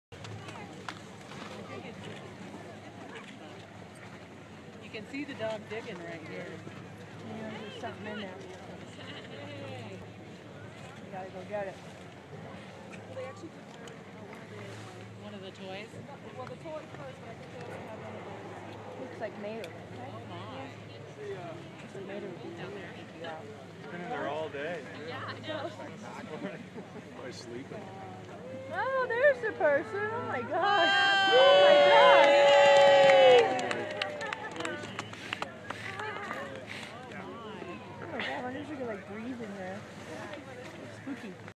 Telluride Ski Patrol Avalanche Dog Demo
In celebration of their 40th birthday, Telluride Ski Resort has hosted 40 days of Telluride, featuring 40 days of different events for the last 40 days of the ski season 2012/2013. On Tuesday, March 19th the Telluride Ski Patrol offered a patrol dog demonstration to an enthusiastic crowd at the bottom of lift 4. Here, on of the three dogs on site located a snow-covered ski patroller.